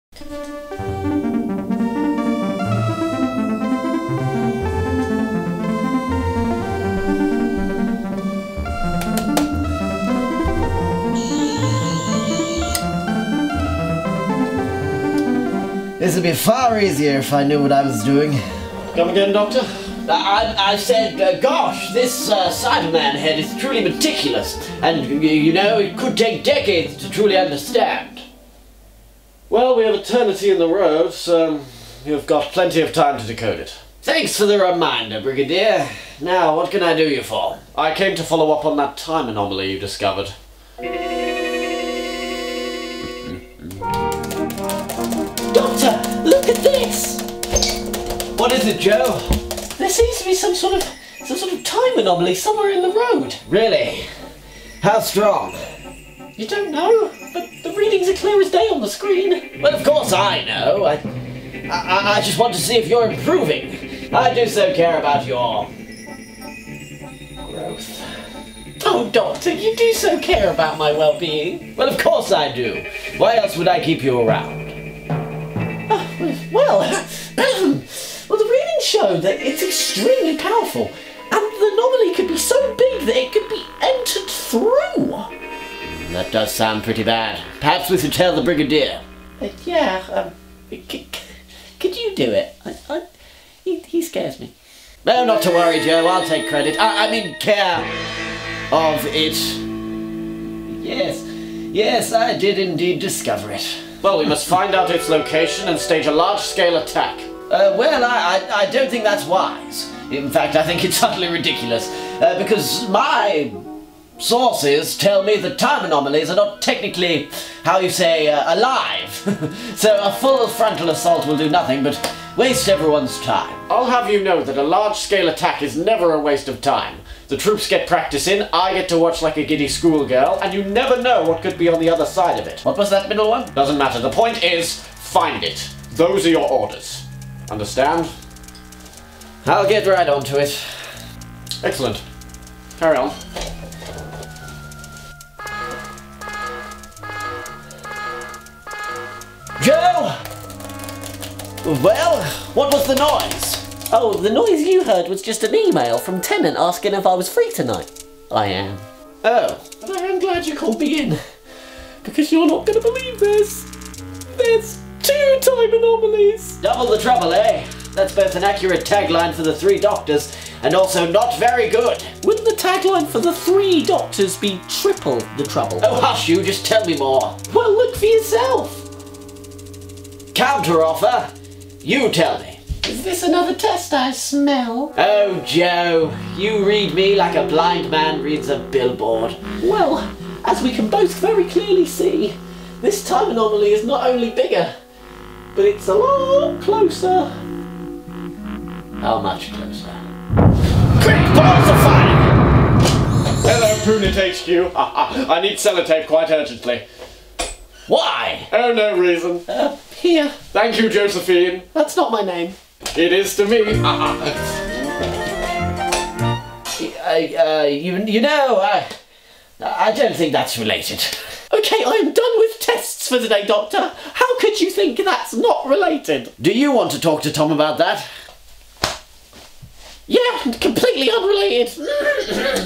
The Time Anomalies - Mini-Sketch (3/13)
Jon Pertwee lucks his way through another day at Doctor Who Roads UNIT HQ with his friends The Brigadier and Jo Grant, where they try and decipher these strange Time Anomalies.

The Doctorwhoguy
DWG Productions
2020